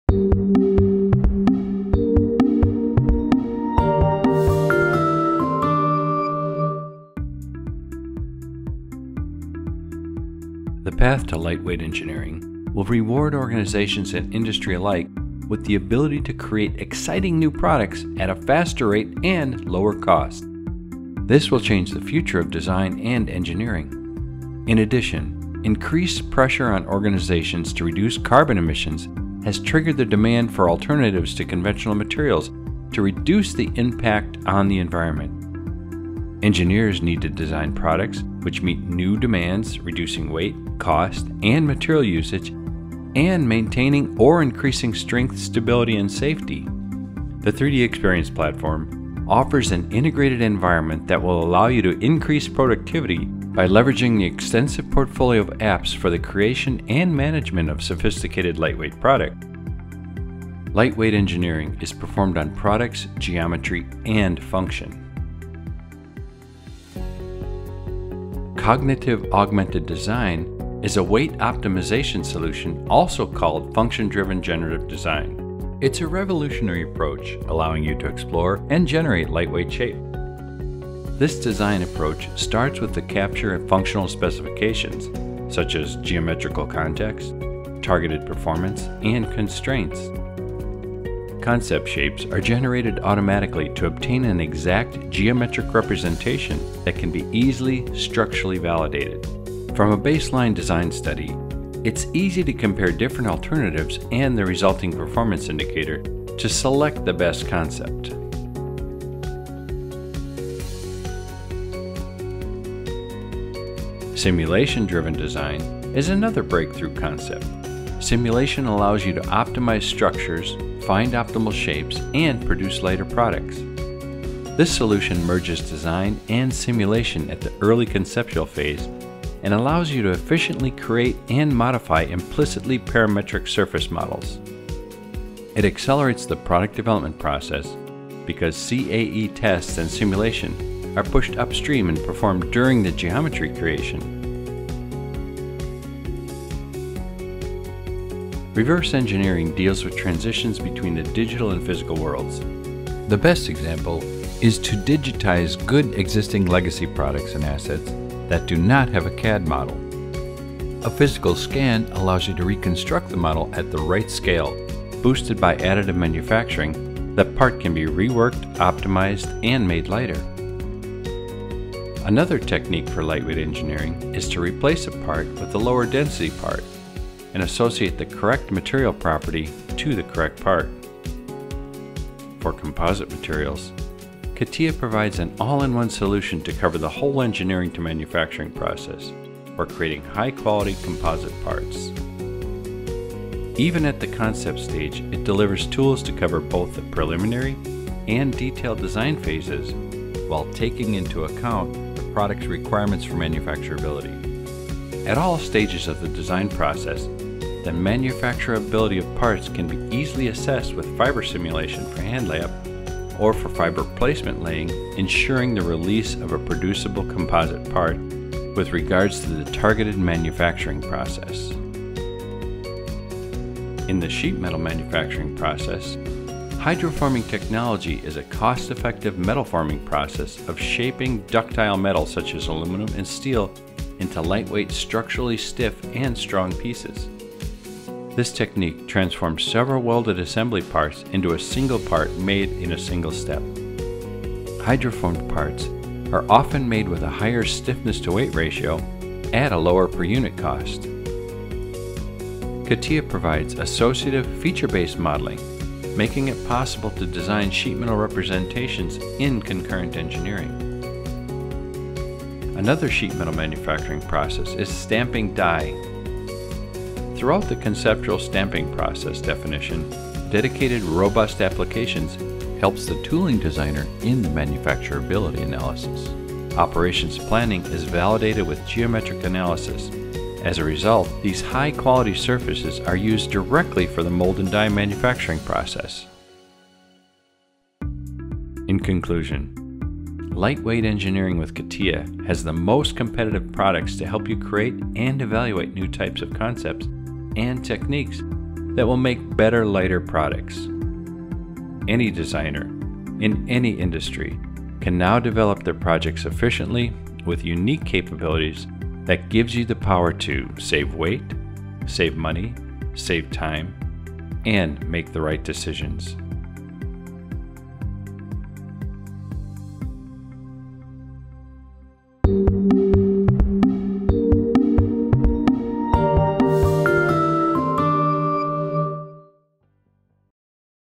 CATIA Lightweight Engineering
The path to lightweight engineering will reward organizations and industry alike with the ability to create exciting new products at a faster rate and lower cost. This will change the future of design and engineering.